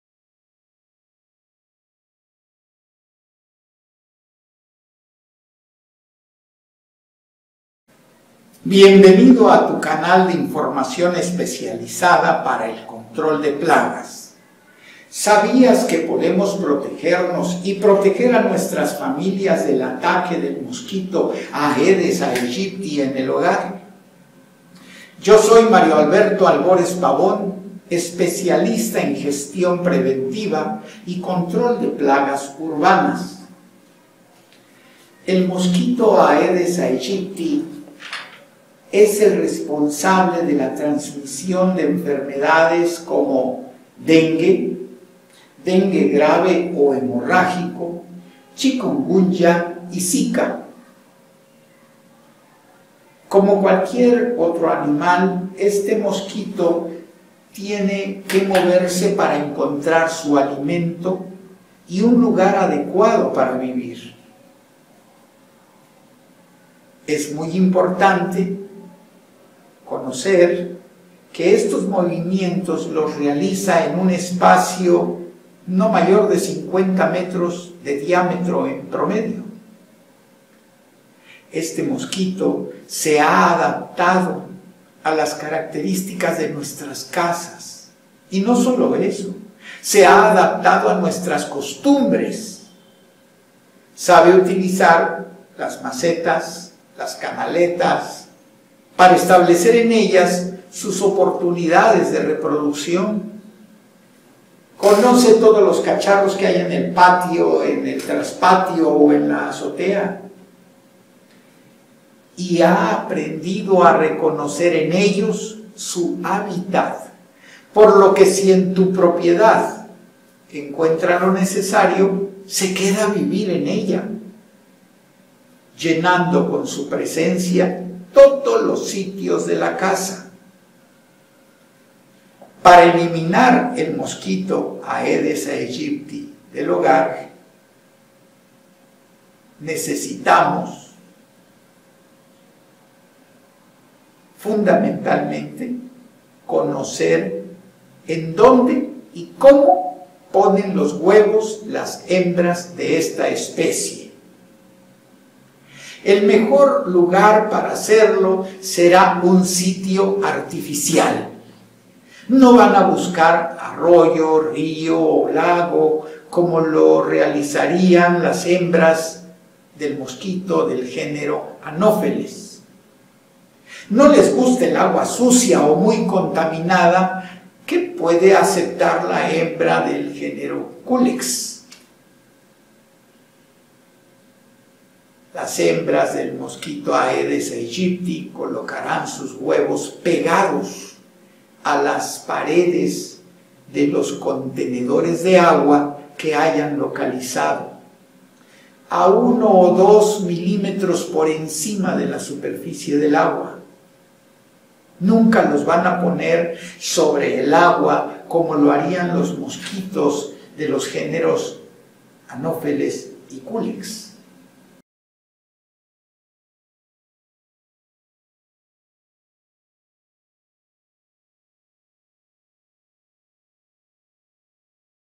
01 Mosquito Aedes aegypti (parte 1 de 3)
Un vídeo con la información necesaria para conocer al mosquito Aedes aegypti y prepararte para eliminarlo.
Protegerte y proteger a tu familia del ataque de este insecto ya podrás hacerlo.